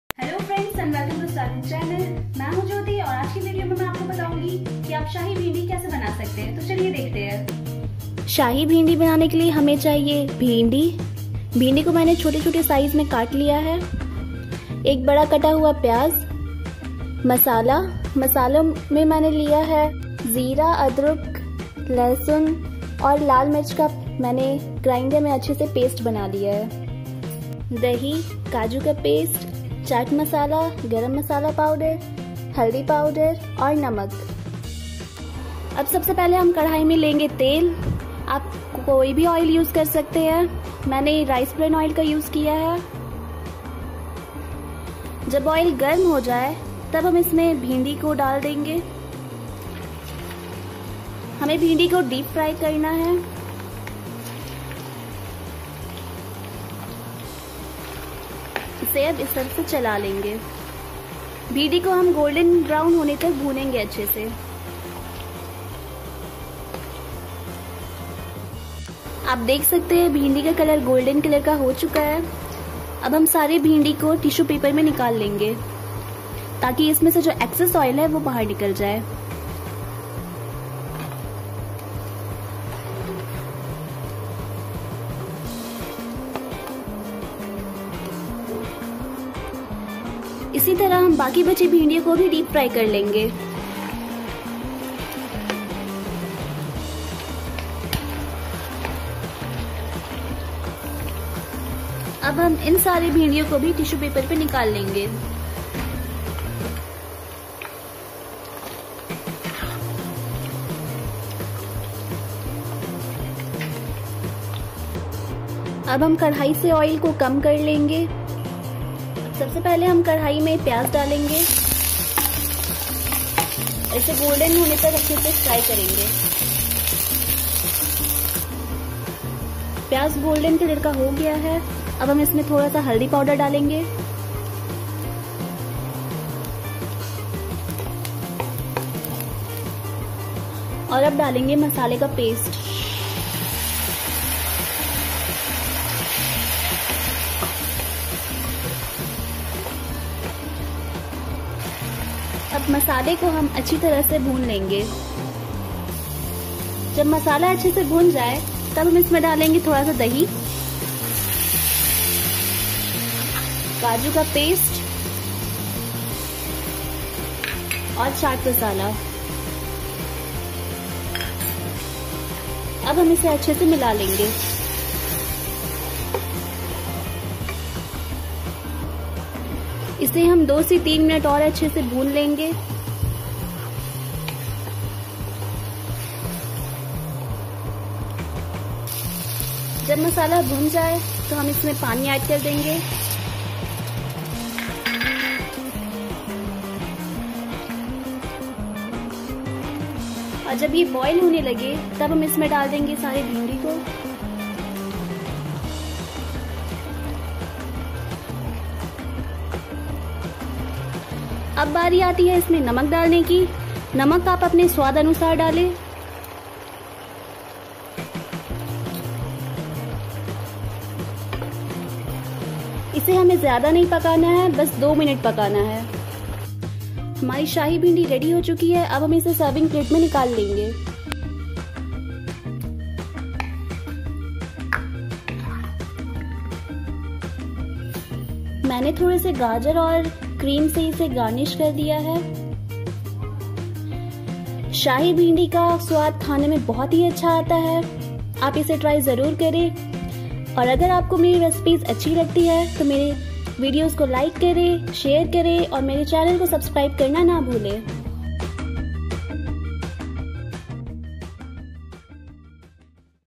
Shahi Bhindi |Restuarant style at home |Star COOK
Hello friends, Welcome to Star COOK channel. In this video I'm going to show you how you can make Shahi Bhendi at home.

Ingredients required:
1) Bhendi
2) Curd
3) Chopped onion
4) Garam masala powder
5) Chat masala
6) Paste of kaaju
7) Oil
8) Salt
Try this recipe and share your feedback by commenting below.